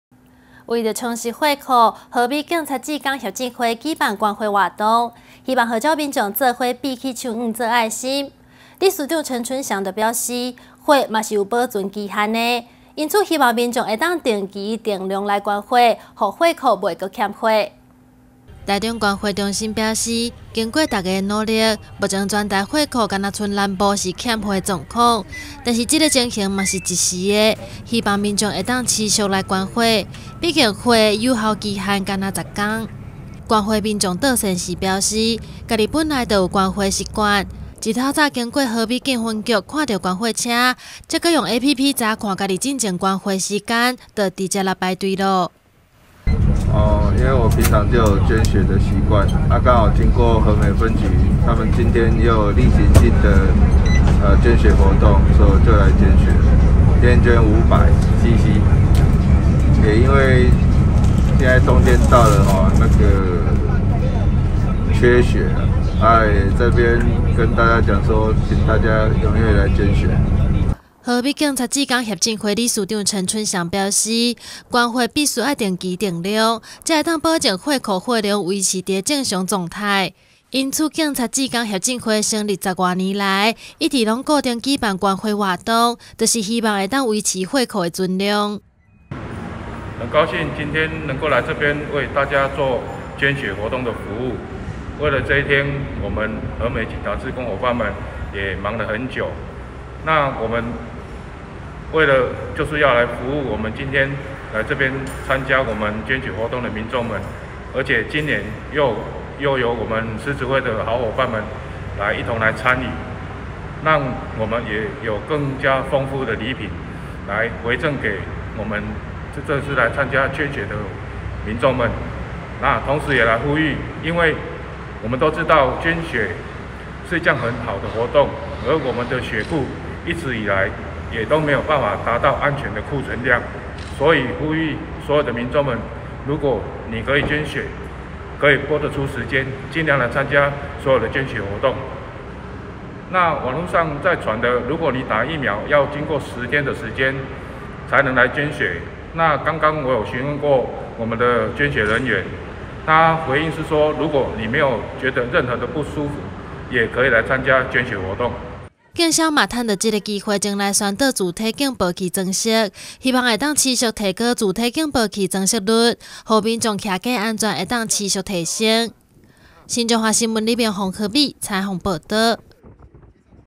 新彰化新聞20211019 充盈中部血庫 和美警察志工協進會辦捐血
為了讓血庫得以不斷有新血替換，和美警察志工協進會舉辦捐血活動，希望號召眾人一起挽袖，理事長陳春祥表示，捐出的血僅有數十天的保存期限，因此希望民眾要養成定期捐血的習慣，不僅有益身體健康也可以讓血庫定期定量進帳不缺血。